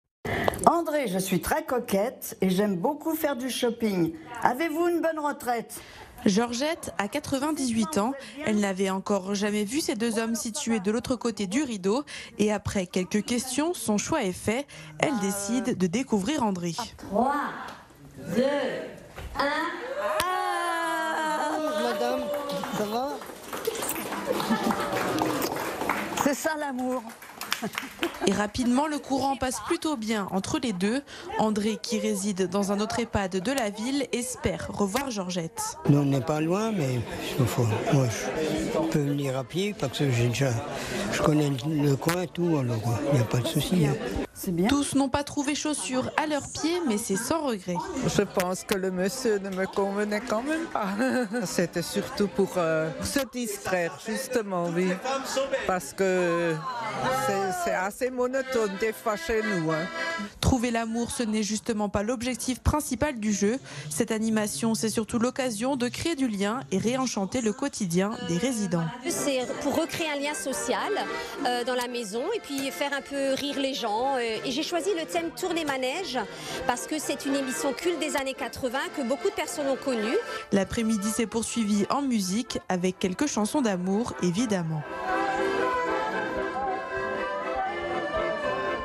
Mulhouse: un Ehpad célèbre la Saint-Valentin en jouant à "Tournez Manège"
Pour la Saint-Valentin, un Ehpad de Mulhouse a organisé un "Tournez Manège" afin que ses résidents trouvent l'amour.